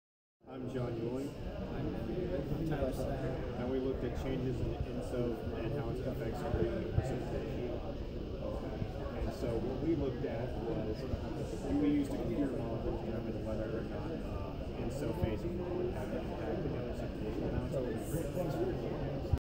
Senior Capstone Project - Ben Davis, Tyler Snider, and John Uehling
We challenged our graduating seniors to present the research they've been working on for the better part of a year in under a minute!